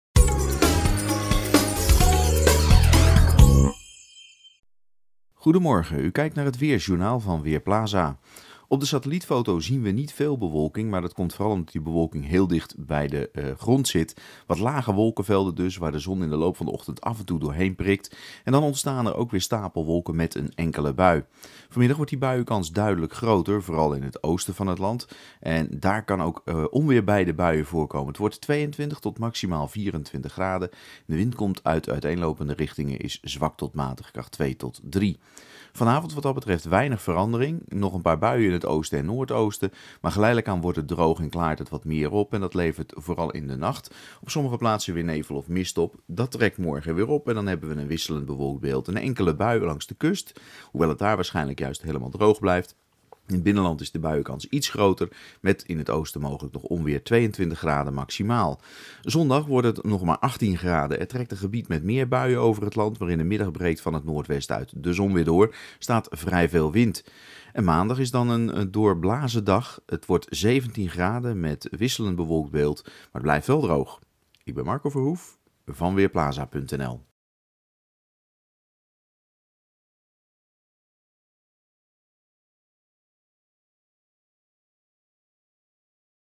Weerbericht vrijdagochtend: Enkele buien
In de loop van de ochtend breekt de zon door, maar verspreid over het land kan ook een regen- of onweersbui vallen, met de grootste kans in het oosten en noordoosten. Het wordt 21 tot 24 graden bij weinig wind. Komende nacht is opnieuw mist mogelijk. Verder vallen er in het weekend verspreid over het land buien, afgewisseld met zon. Morgen wordt het 20 tot 23 graden, zondag 17 tot 19 graden. De nieuwe werkweek start droog.